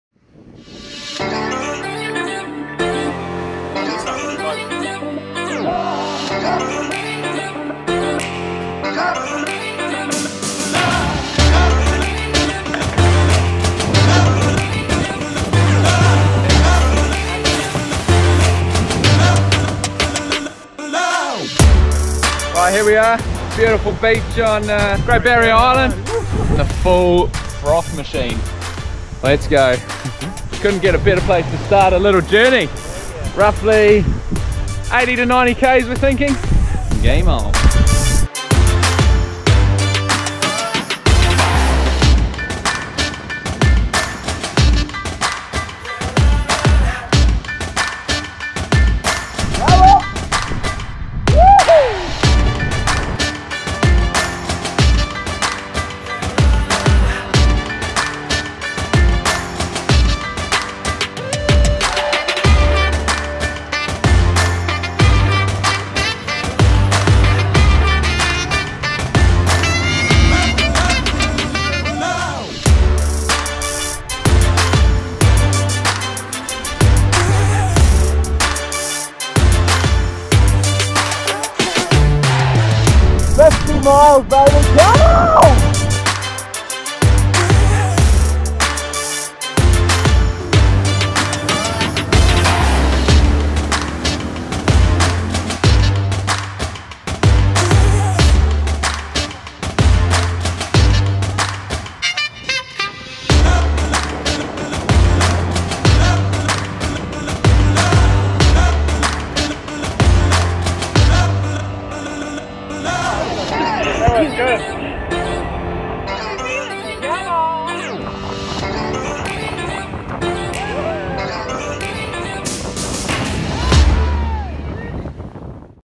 Episode 3 : Wing Mission - Great Barrier Island to Auckland
Phil and friends execute an absolutely epic foiling mission in the Hauraki Gulf of Auckland, New Zealand. Setting out from the Island of Great Barrier (Aotea), the mission is to foil with only board and wing, unassisted all the way to Auckland city. With over 110km (56nm) of distance, even with favourable weather conditions, this is not a mission for the faint hearted and requires meticulous planning, specific training and huge levels of endurance, both physical and mental. To see the result, have a look at the video below.